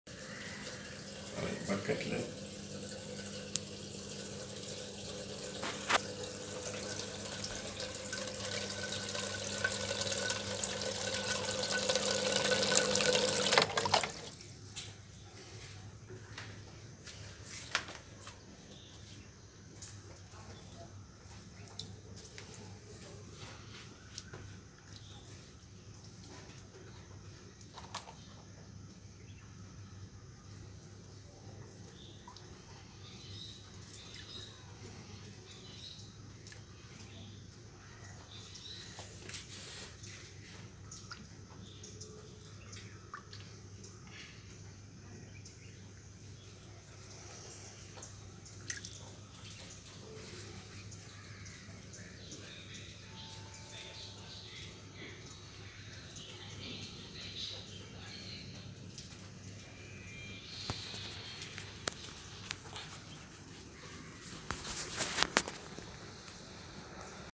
Tilapia in Flat..!!! 😂😂😂😁😁😀
Tilapia farming in flat balcony. edible fish grows in balcony. entertainment for family. Kochi city balcony fish grows in big plastic bucket. do it yourself hobbies for kids. only one old bucket and tilapia fish seeds needed.